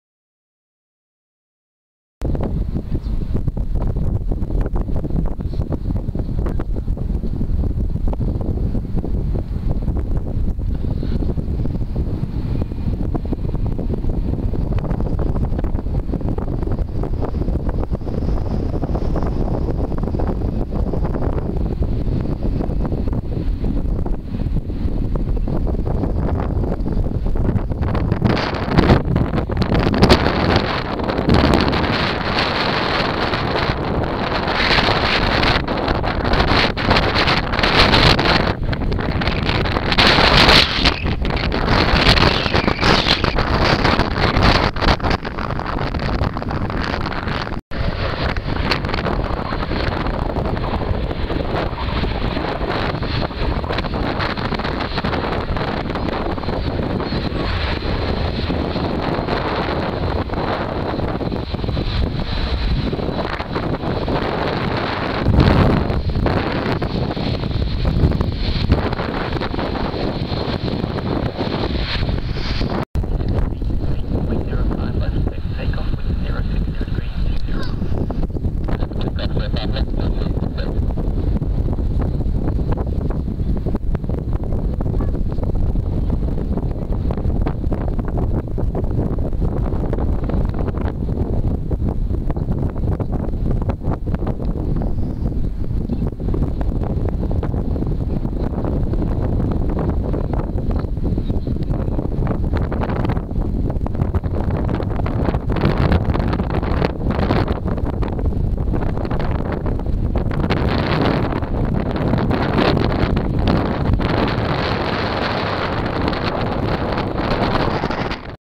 Flybe De-Havilland Dash 8-Q400 at Manchester Airport 28th April 2012 (FULL HD)
The Aircraft that isnt really recorded much at Manchester! Cant wait to fly on this in the summer! The Flybe De-Havilland Dash 8-Q400 landing, taxxing and taking off on runway 05L at Manchester Airport on the 28th April 2012. In FULL HD